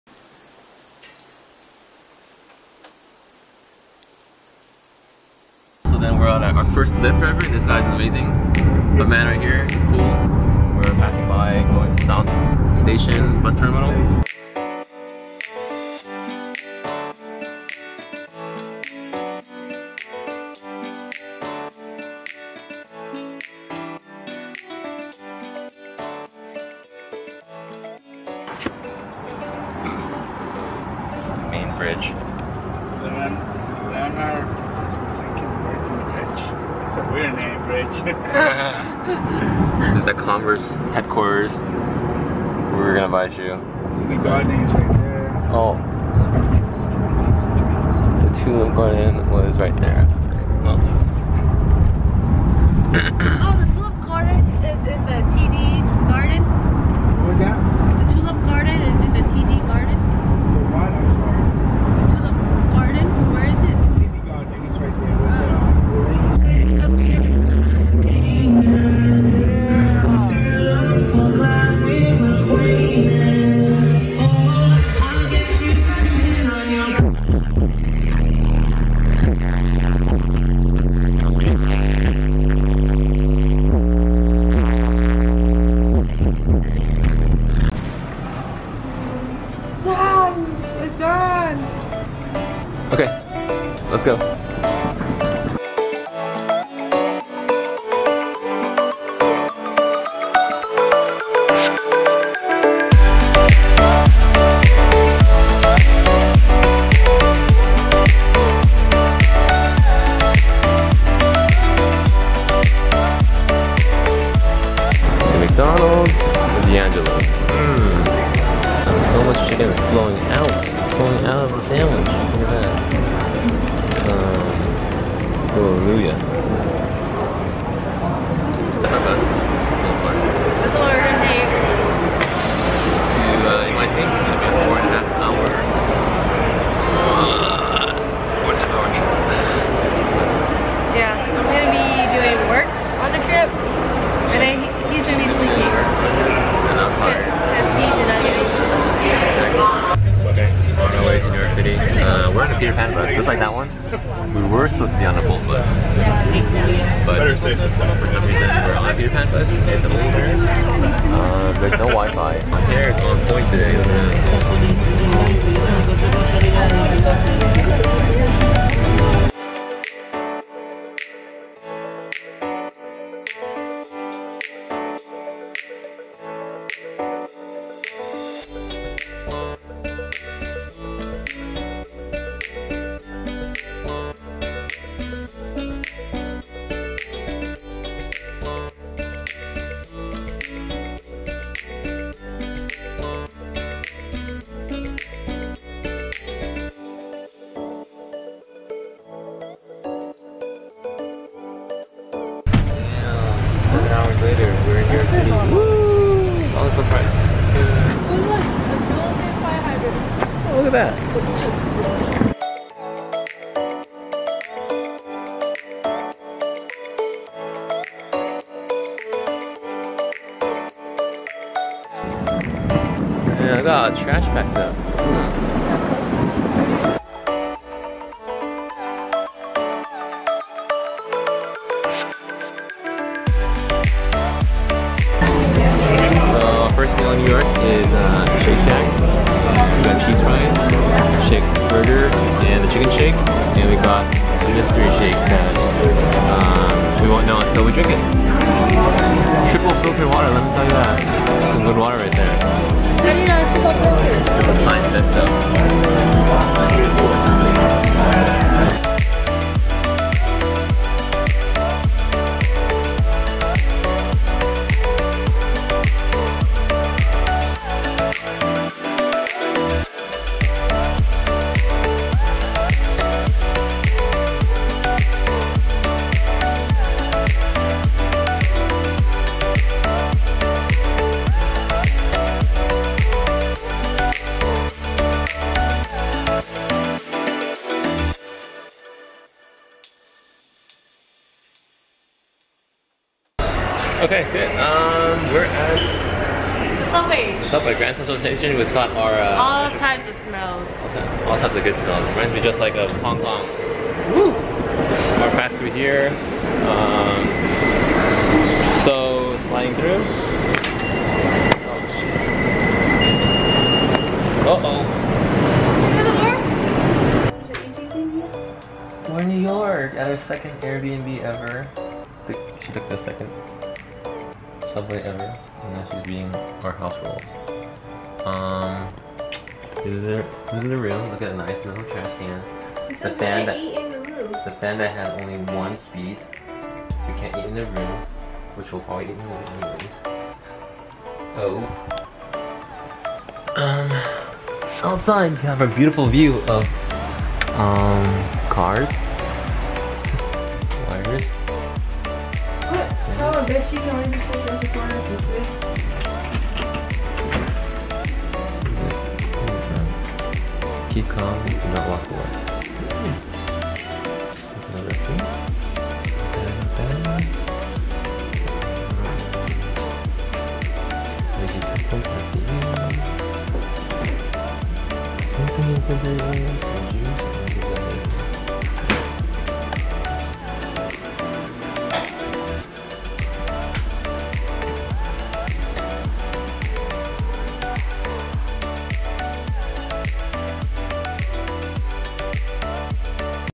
Are we there yet? Bolt Bus Fail - Best Lyft Ride? - Day 2 - East Coast Trip
Checked out of our first Air BnB in Boston and headed to the Big Apple! Still not much sleep but onward!

First Bolt Bus or nah?
First Lyft Ride
First Shake Shack